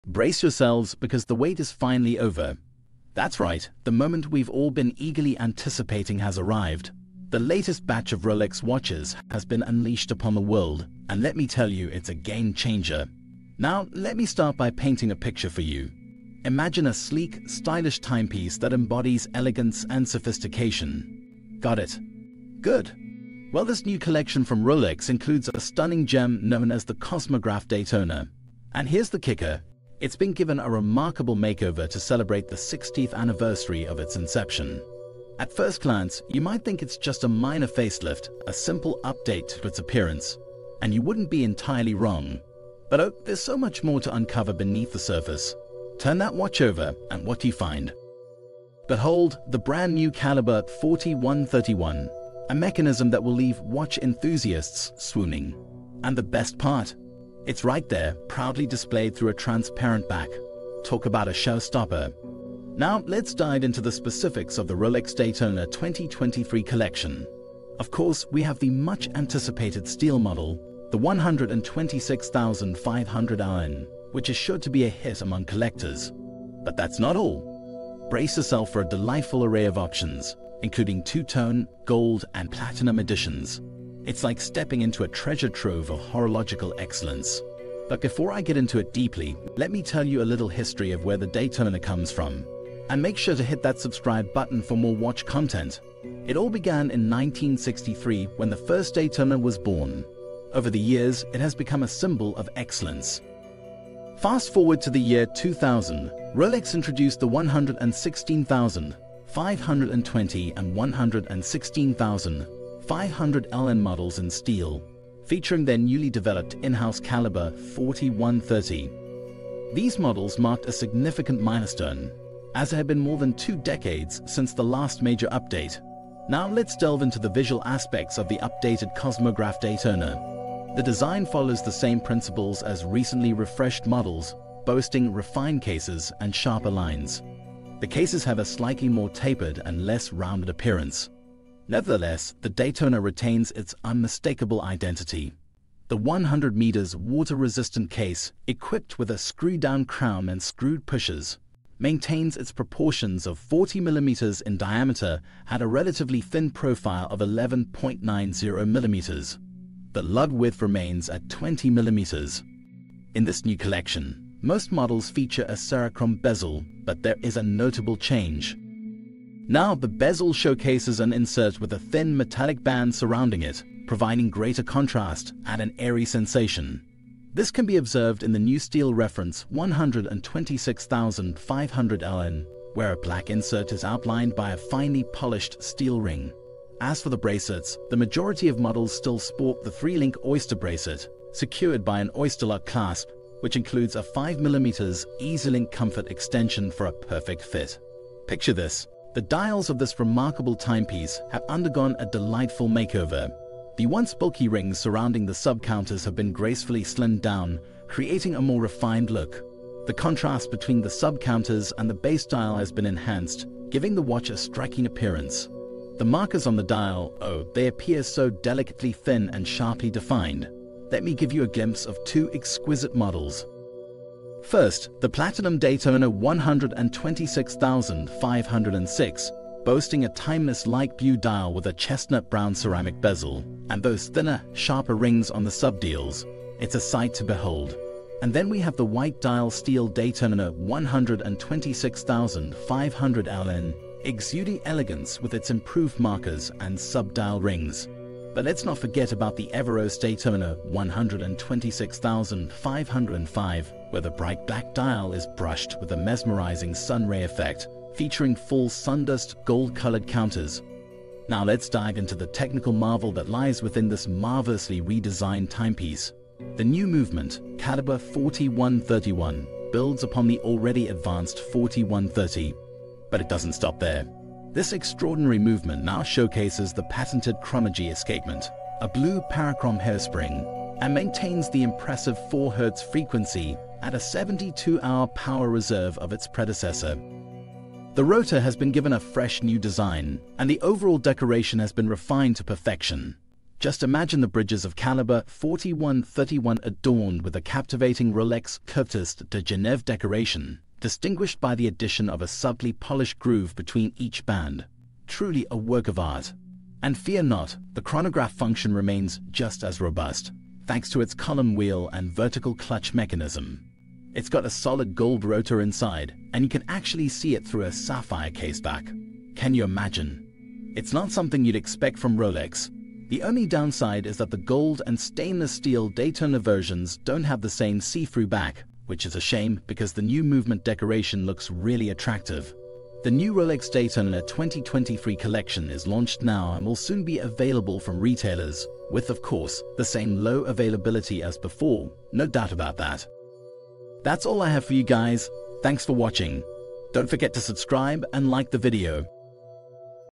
The New Rolex Daytona Releases 2023 – New Daytona Updates
In this video, we're taking a look at the new Rolex Daytona releases for 2023. We're also discussing the new Daytona updates and how they might impact collectors and watch enthusiasts.

If you're interested in watches or horology, be sure to watch this video! We're discussing the latest news and releases from Rolex, including the new Rolex Daytona releases for 2023. So whether you're a collector or just interested in what's new, be sure to watch this video!